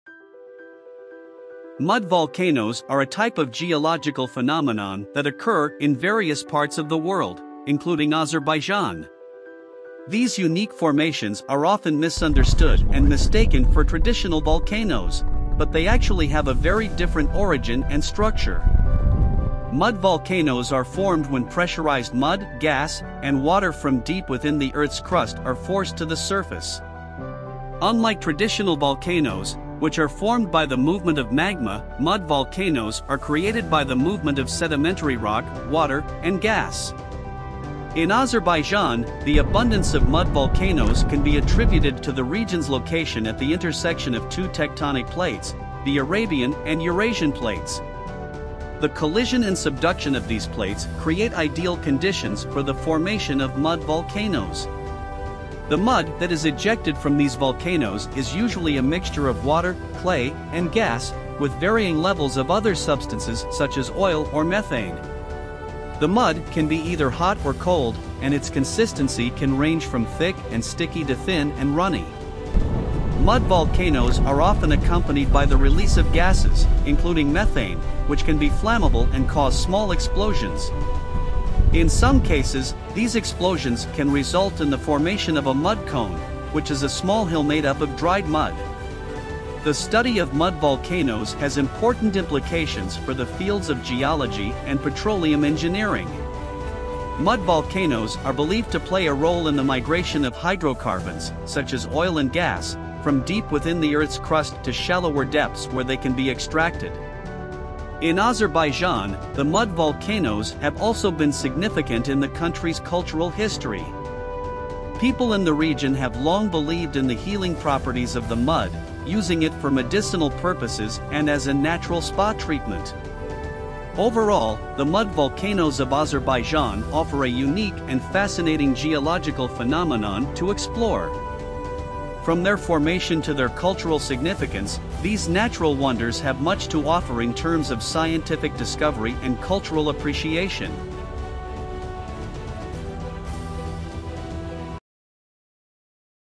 Azerbaijan's Mud Volcanoes: Explore the Fascinating
Azerbaijan is home to a unique geological phenomenon - the Mud Volcanoes. These are not your traditional volcanoes, but rather formations that are created by the movement of sedimentary rock, water, and gas from deep within the earth's crust. With over 400 mud volcanoes, Azerbaijan boasts the largest concentration of these natural wonders in the world. From the healing properties of the mud to their significance in petroleum engineering, these volcanoes offer a fascinating glimpse into the geological and cultural history of Azerbaijan. In this article, we'll explore the formation, characteristics, and significance of these mud volcanoes, as well as why they're an essential stop on any nature-lover's itinerary.